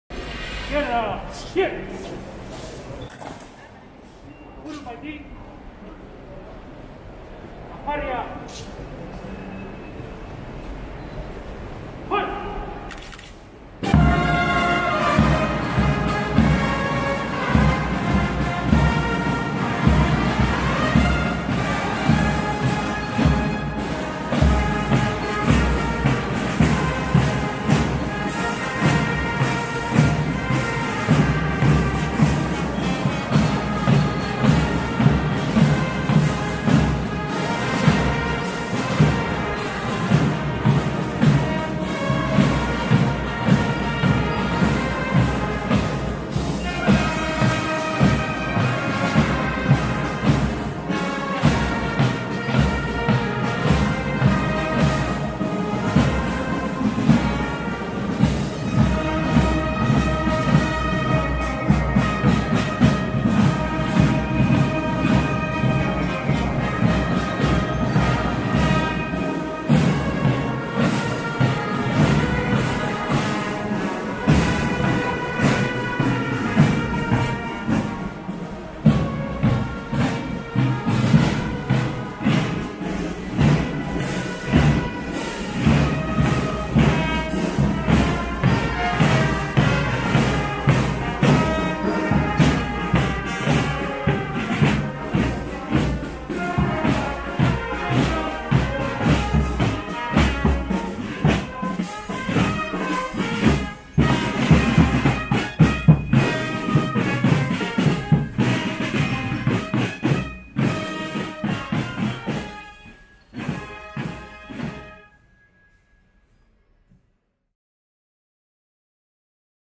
156 ANIVERSARIO DE CURUPAYTI
A 156 años de esta batalla, compartimos este recuerdo de la Fanfarria Militar "Alto Perú" del Regimiento de Granaderos a Caballo "General San Martin" en la Estacion Retiro donde brinda su mas sentido homenaje a los heroes que "inmolaron sus vidas en los campos de Tuyuti, Humaitá y Curupaity"
Porque defender a la Patria es actuar con heroísmo, con valentía y generosidad suprema. Es ofrecer la propia vida a las generaciones futuras, buscando que los argentinos vivan y sueñen con un país justo, libre y soberano.